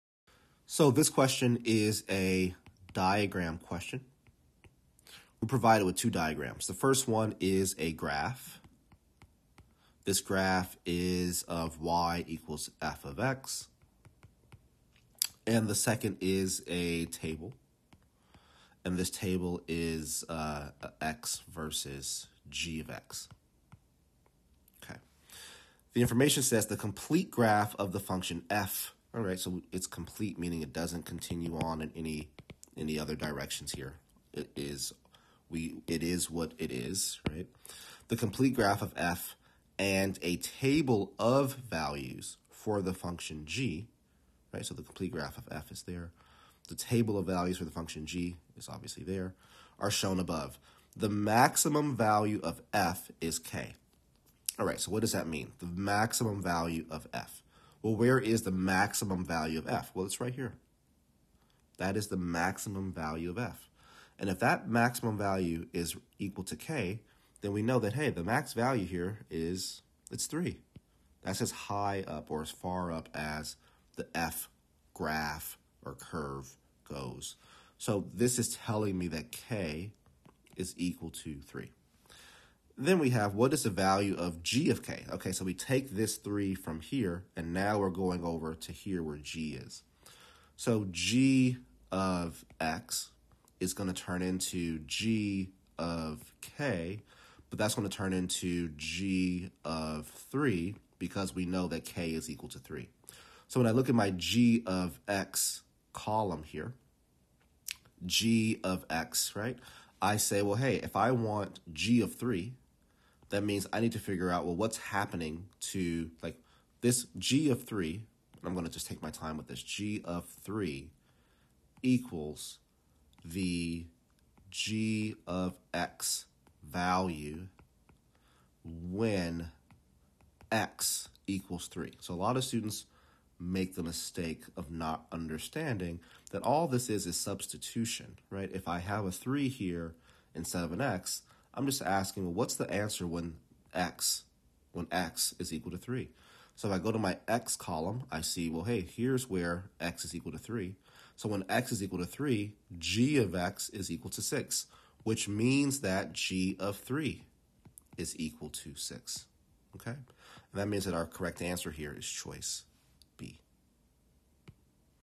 The complete graph of the function f and a table of values for the function f and a table of.....
Official SAT Practice Test 8, Section 4, Question 30:

The complete graph of the function f and a table of values for the function f and a table of values for the function g are shown above. The maximum value of f is k.  What is the value of g(k) ?